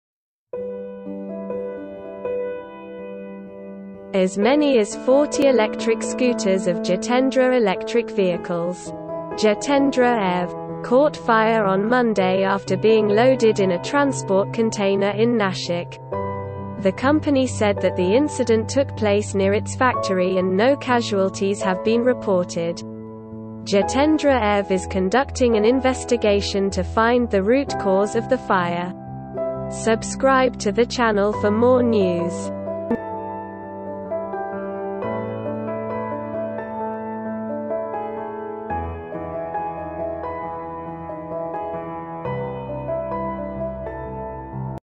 40 e-scooters of Jitendra EV catch fire in a truck in Nashik, no casualties reported
fore more NEWS. ♥ ♥ ♥ ♥ ♥ ♥ ♥ ♥ ♥ ♥ ♥ ♥ ♥ ♥ ♥ ♥ ♥ ♥ ♥ ♥ ♥ ♥ ♥ ♥ ♥ ♥ As many as 40 electric scooters of Jitendra Electric Vehicles (Jitendra EV) caught fire on Monday after being loaded in a transport container in Nashik. The company said that the incident took place near its factory and no casualties have been reported. Jitendra EV is conducting an investigation to find the root cause of the fire.